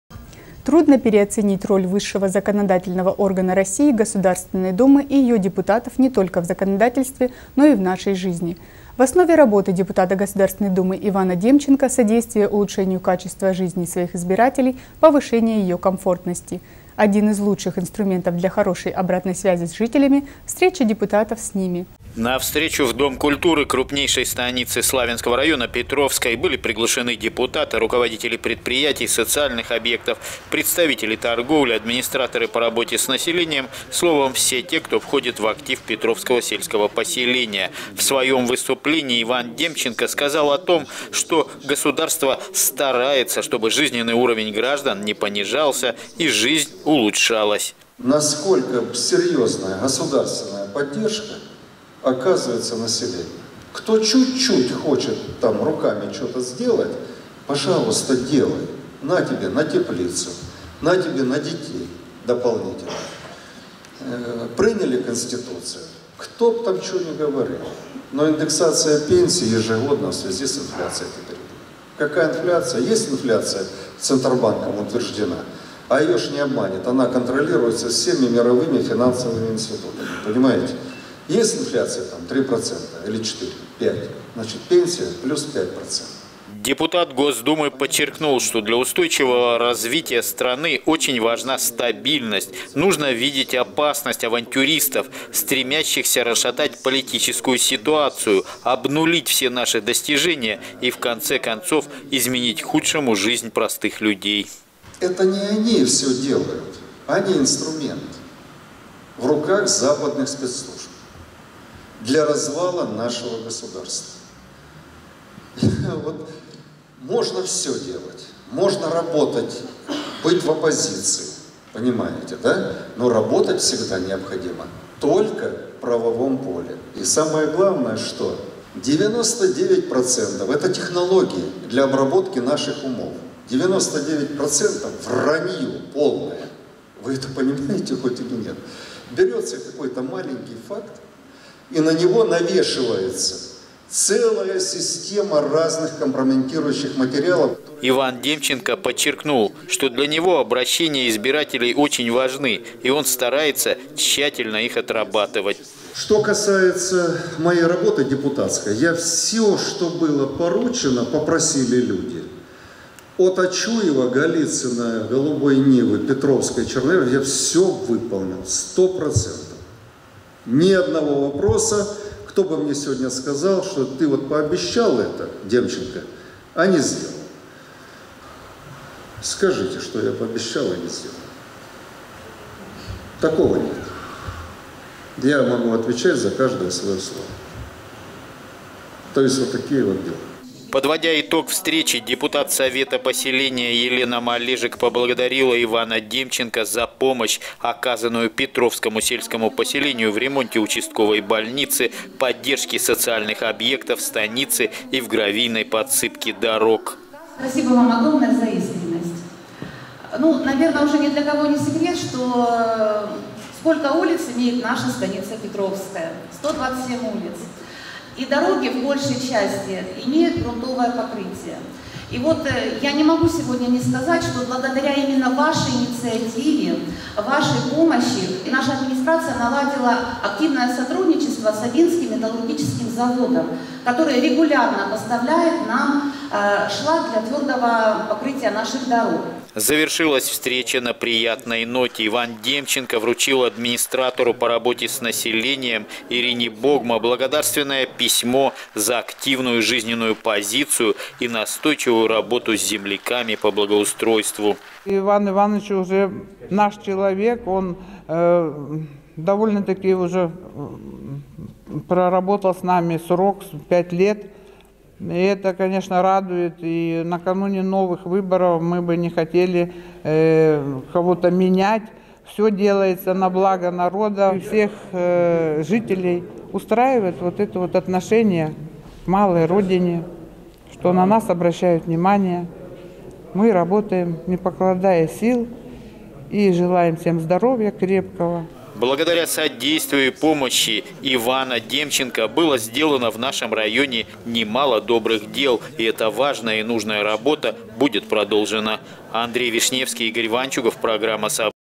Встреча с депутатом ГД РФ Иваном Демченко
Трудно переоценить роль  высшего законодательного органа России – Государственной Думы и ее депутатов не только в законотворчестве, но и в нашей жизни. В основе работы депутата Госдумы Ивана  Демченко – содействие улучшению качества жизни своих избирателей, повышение  ее комфортности.  Один из лучших инструментов  для хорошей обратной связи  с жителями – встречи депутата с ними. На одной из них мы и побывали